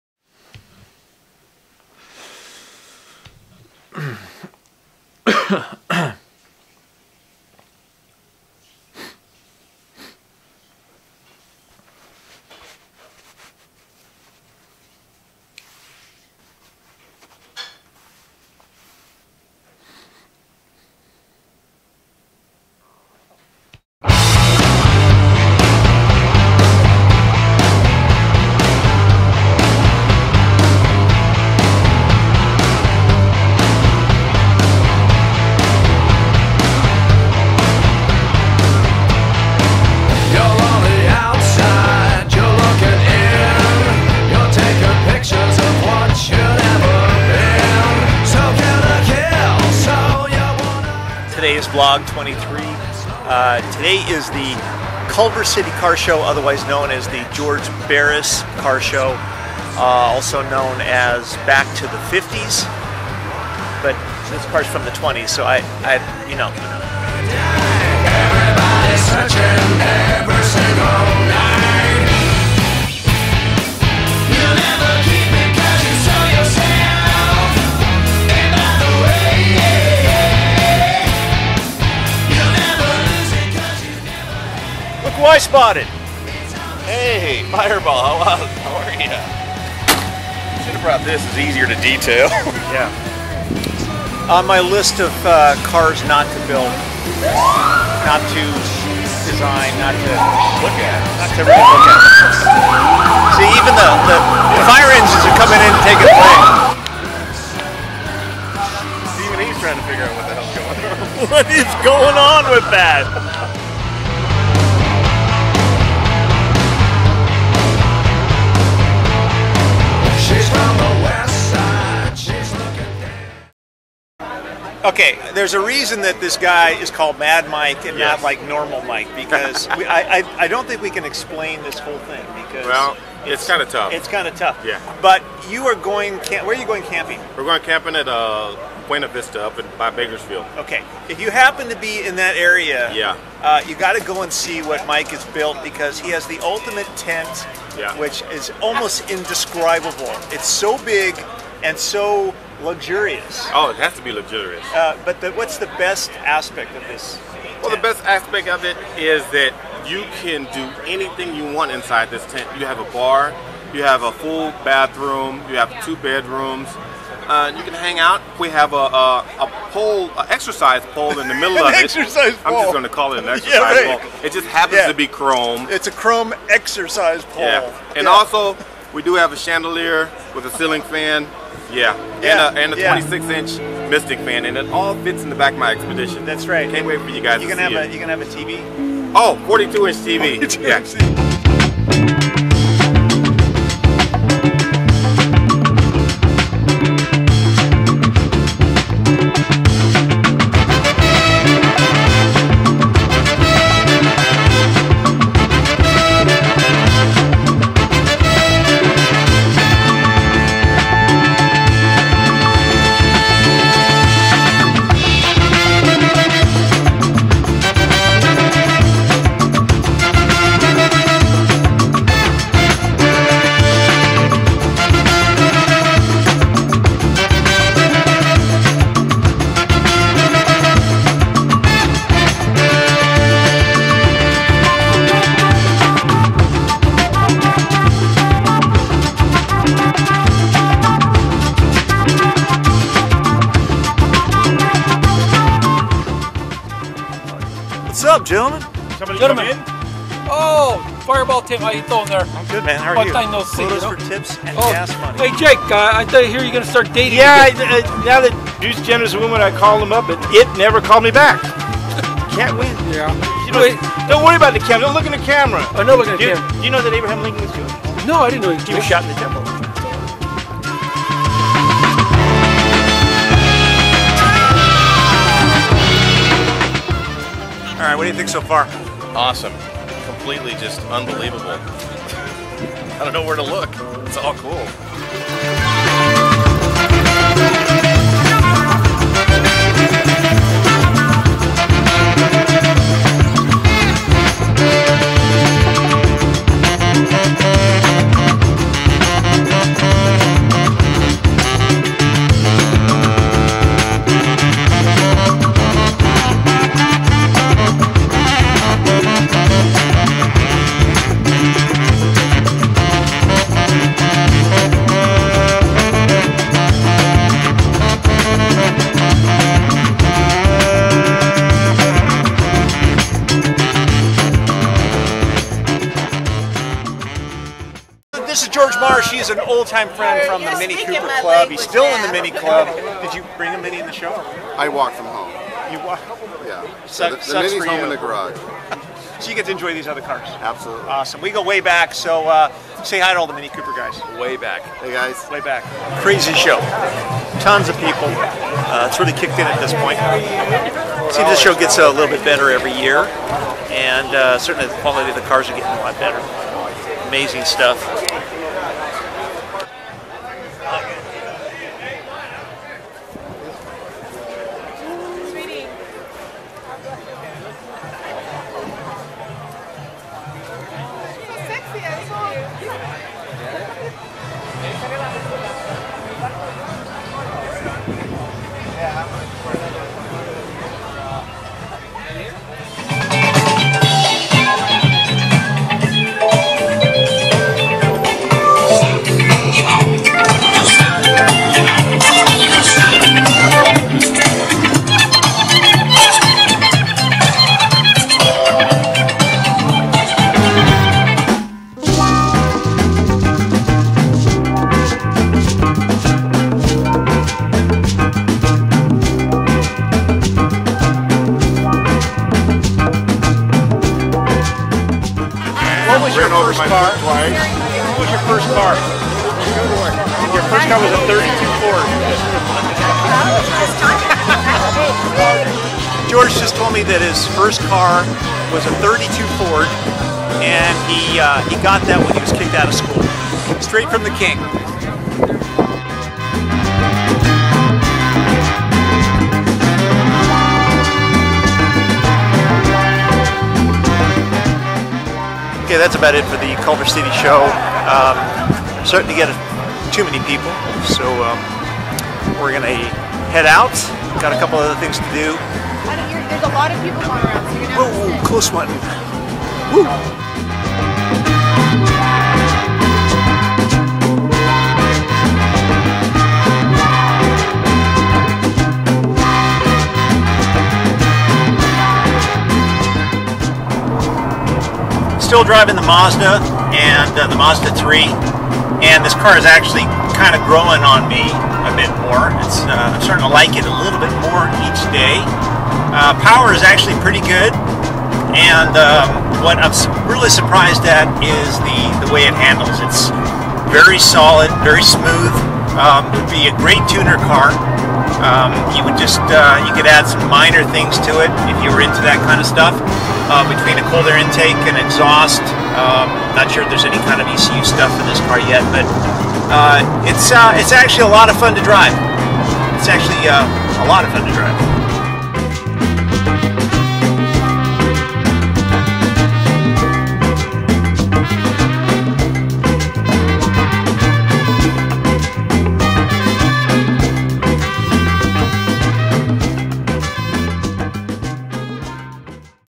Wild & Wacky GEORGE BARRIS Hearse - Fireball Malibu Vlog 23
Wild & Wacky GEORGEBARRISHearse - Fireball Malibu Vlog 23: This episode... Fireball heads to the Culver City GeorgeBarris Car Show. Guest: Mad Mike from "Pimp My Ride."